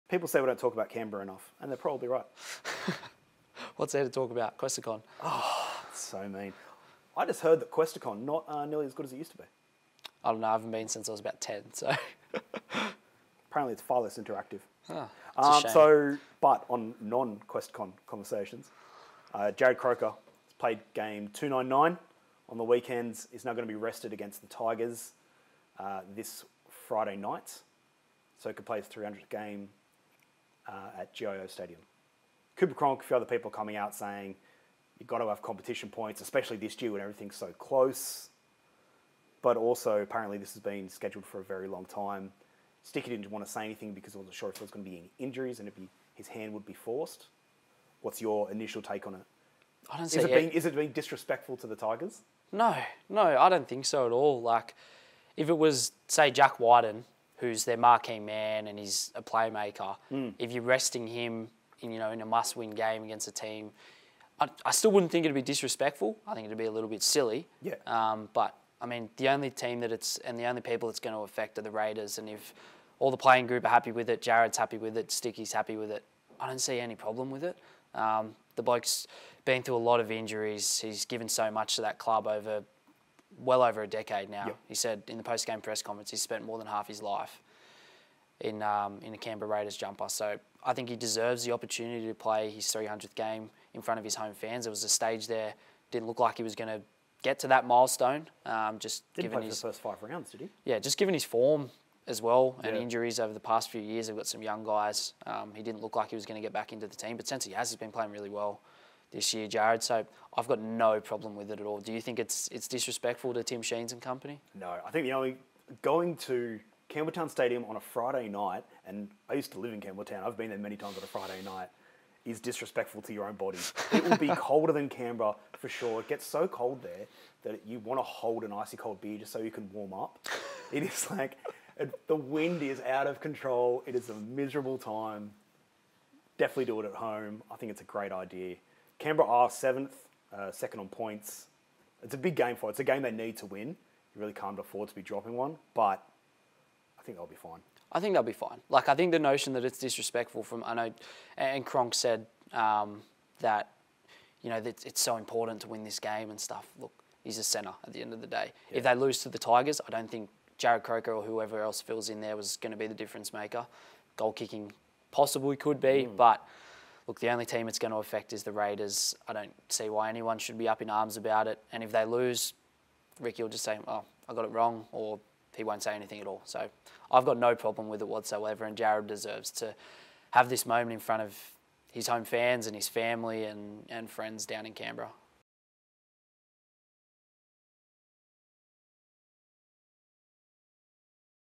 Jarrod Croker's 300th NRL game isn't about disrespect
Canberra Raiders stalwart Jarrod Croker will join the exclusive 300 NRL game club when he next walks onto the field. He should be able to choose whether he does it in front of his home crowd or against the Wests Tigers in Campbelltown.